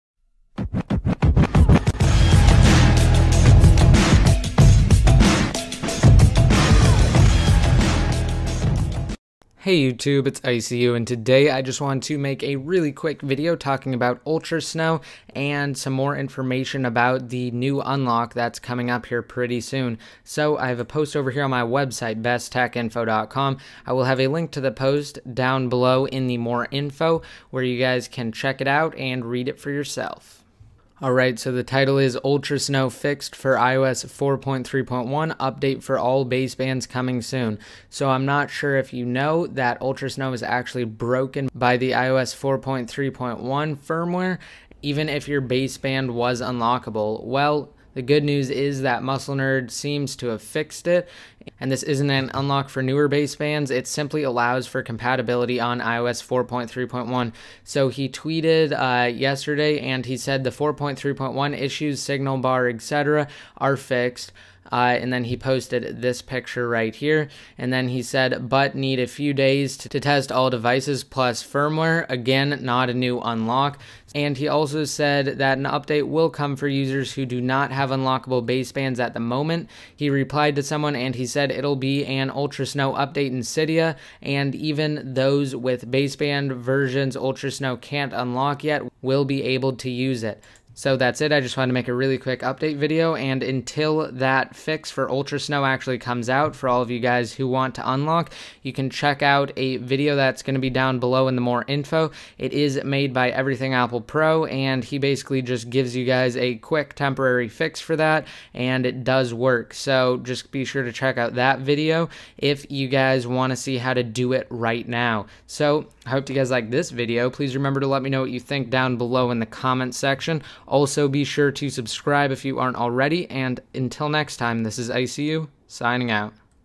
iOS 4.3.3/4.3.1 Unlock - Ultrasn0w Update
Here is the deal, Ultrasn0w will be updated soon to fix iOS 4.3.1 compatibility issues. It looks like we could also see an update (thats not too far off) to incorporate the basebands that have yet to be supported!

Until the update is out, watch Everythingapplepro's video to show you how to re-enable compatibility for iOS 4.3.1 (ON THE ALREADY SUPPORTED BASEBANDS - this will not work for the newer ones)